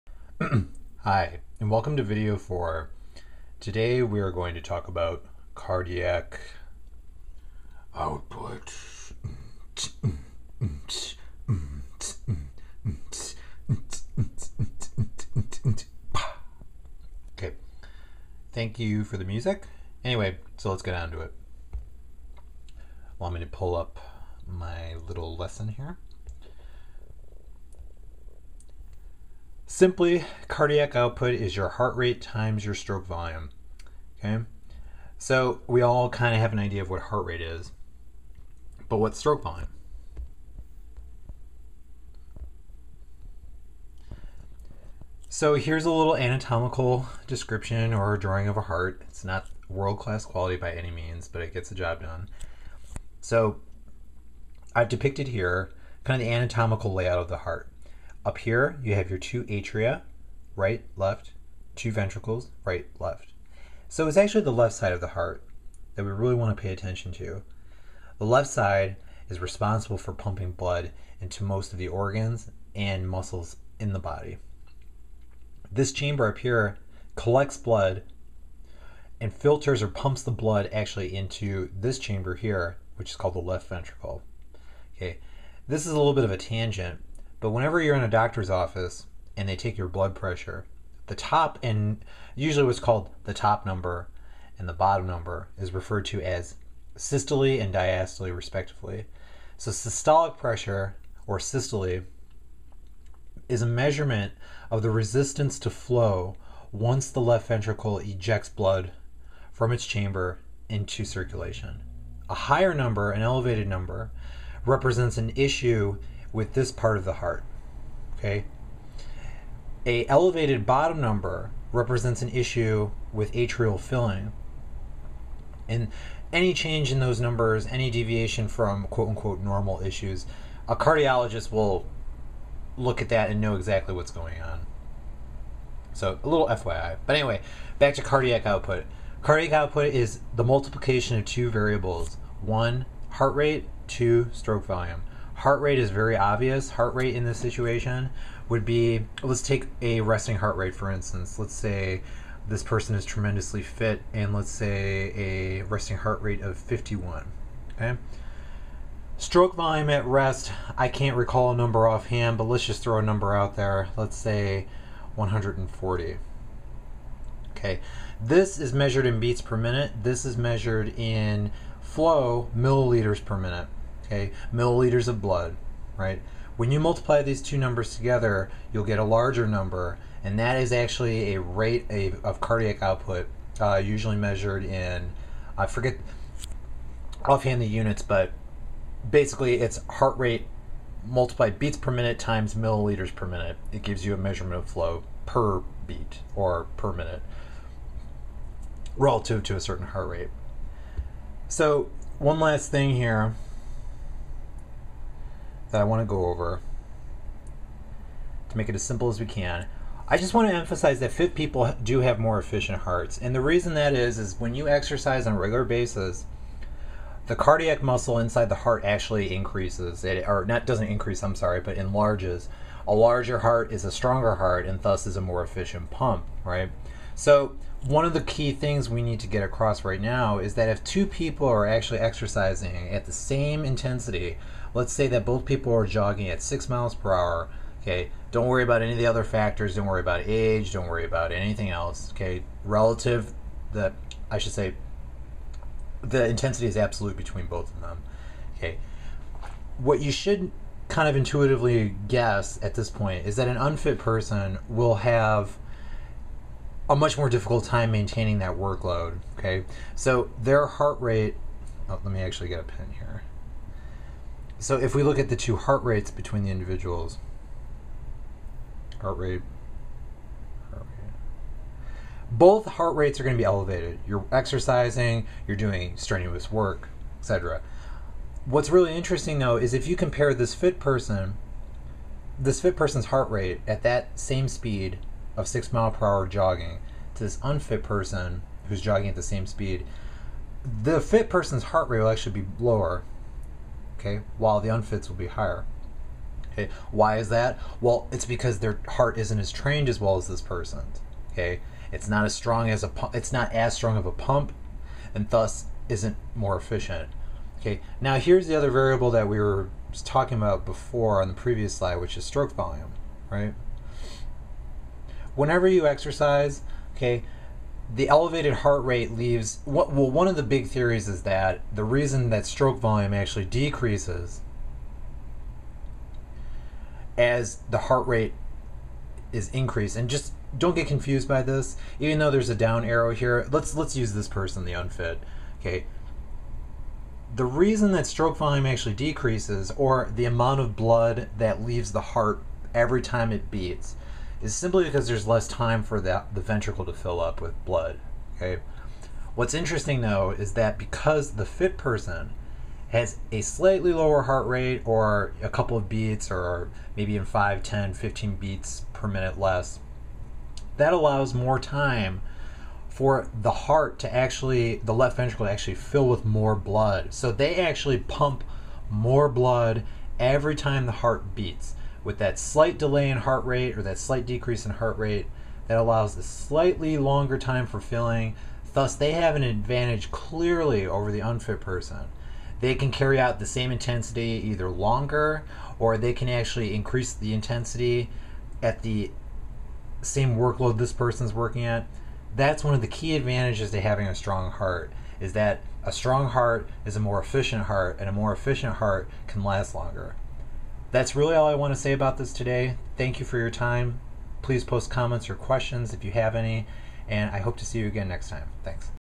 What is Cardiac Output?
A basic introduction to Cardiac Output.  What is it?  How does it relate to exercise?